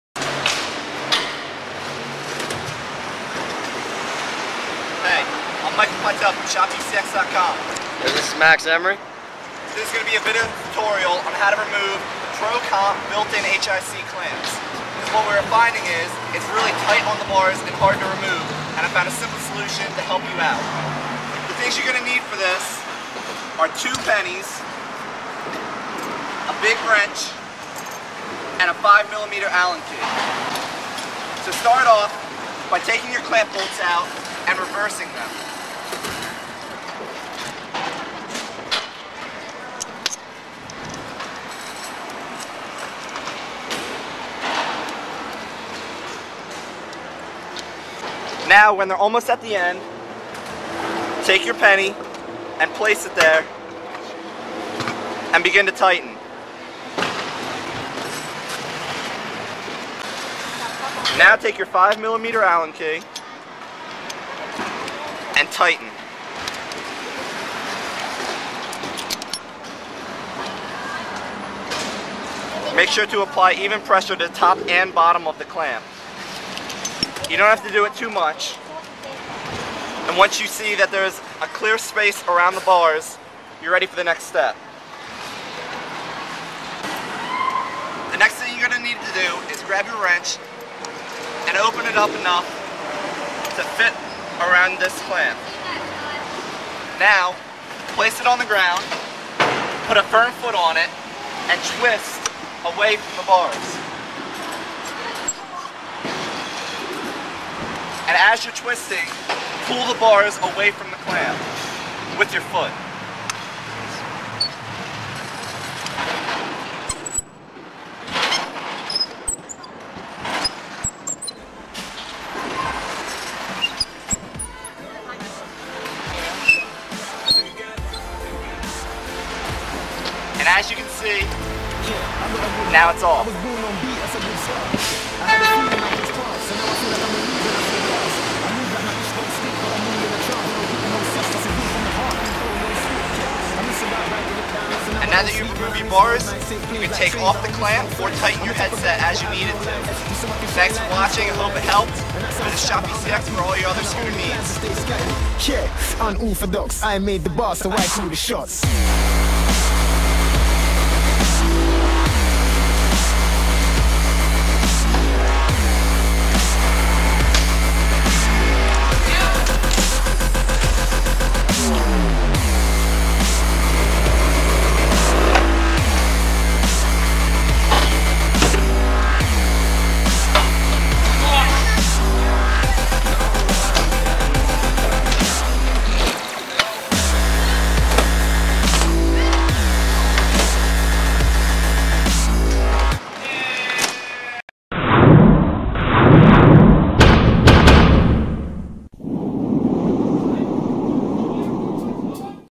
Video tutorial for Help with SZ built in HIC clamps (short vid at end)
This video was to help anyone who is having trouble removing or maintenanceing  their scooter-zone Built in HIC clamps. ECX team rider Michael Pytel explains a simple solution on how to remove your bars from the clamp, because we found they are tight and difficult to remove. (hammering does not work and it damages clamp!)

PS: Pleas also enjoy the short video at the end featuring ECX team riders Brandon Ruhl, Maxx Emrey, and Jon Devrind

The scooter zone Built in HIC clamps are a great design, and perform perfectly, do not think that because of this video that they are faulty. This is simply just a video tutorial just to help make maintenance easier.